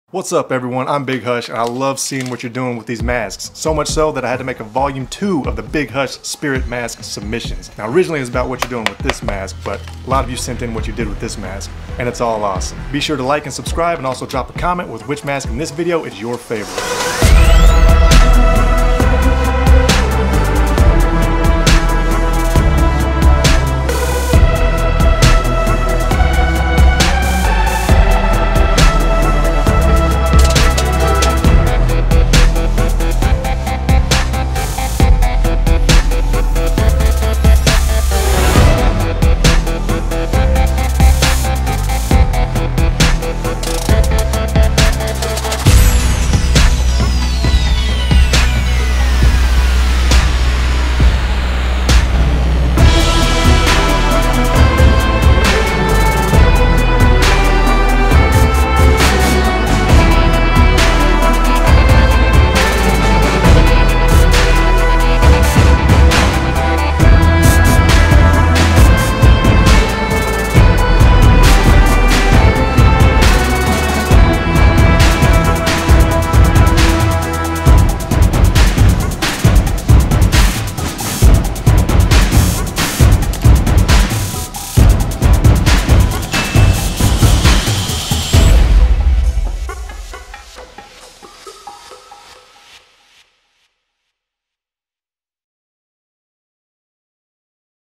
Your Big Hush Spirit Submissions - Volume 2
Thank you all again for the awesome custom mask photos you submitted!